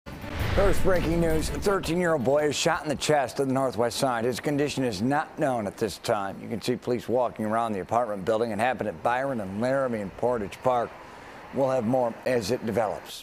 13-Year-Old Boy Shot In Portage Park
A 13-year-old boy was shot and wounded Wednesday evening on the Northwest Side.